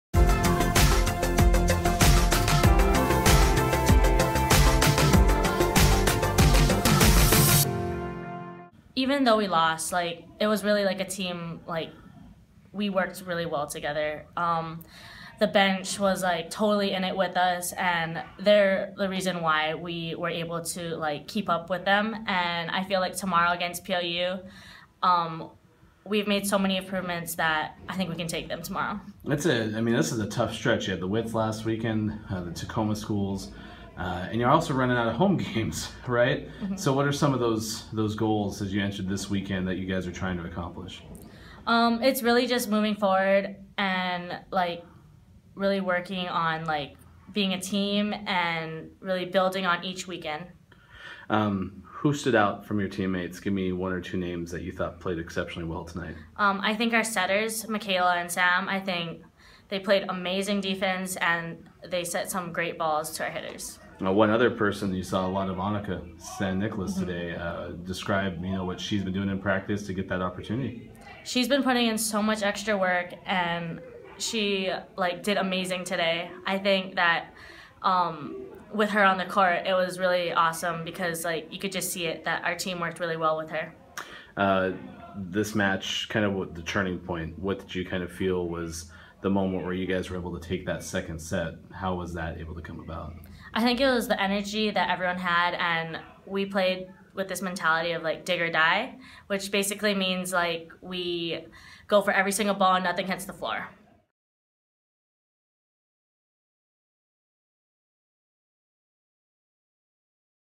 Postmatch: Alexe Navarro - Puget Sound 3, Pioneers - 10.14.16
Following a match with Puget Sound, Lewis & Clark libero Alexe Navarro discussed the result.